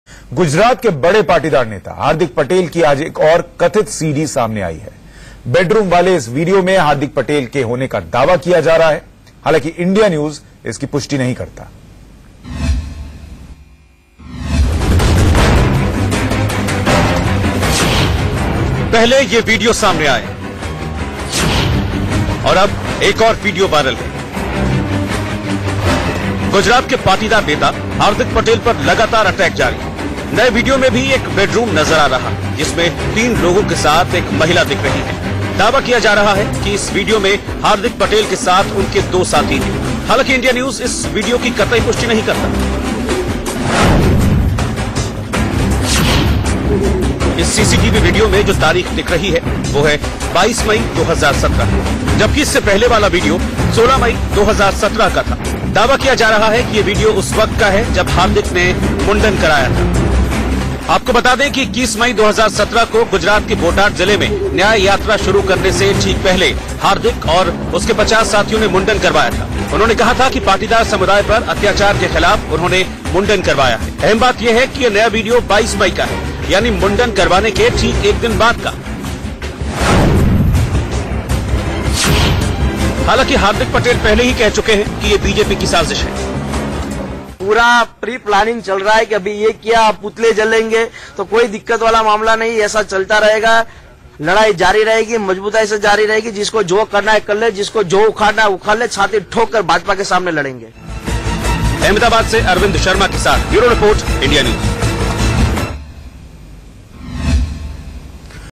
हार्दिक पटेल अश्लील वीडियो: पाटीदार नेता Hardik की एक और CD वायरल- लड़की एक, लड़के तीन | Suno India
पाटीदार नेता हार्दिक पटेल की दूसरी वीडियो वायरल हो गई है. इस वीडियो में हार्दिक जैसा एक युवक दिख रहा है जिसके साथ दो और लड़के और एक लड़की है. लड़कों ने सिर मुंडवा रखी है. लड़की और लड़के ईनर्जी ड्रिंक पी रहे हैं और बात कर रहे हैं और फिर वीडियो के आखिर में हार्दिक और बाकी सारे लोग उस कमरे से निकल जाते हैं. हार्दिक पटेल की लड़की के साथ जारी हुई इस दूसरी सीडी में कोई अश्लील हरकत कैमरे पर कैद नहीं हुई है सिवा इसके कि तीन लड़के और एक लड़की कुछ पी रहे हैं और अंतरंग माहौल में बात कर रहे हैं. जानकारी के मुताबिक ये वीडियो 22 मई का बताया जा रहा है. गौरतलब है कि सोमवार को भी हार्दिक पटेल की दो कथित वीडियो सामने आई थी जिसे हार्दिक ने बीजेपी की भद्दी राजनीति करार दिया था. हार्दिक पटेल ने कहा था कि 'मैं मर्द हूं, नपुंसक नहीं' उन्होंने कहा था कि मेरी छवि खराब करने के लिए करोड़ो रूपये खर्च किए जा रहे हैं.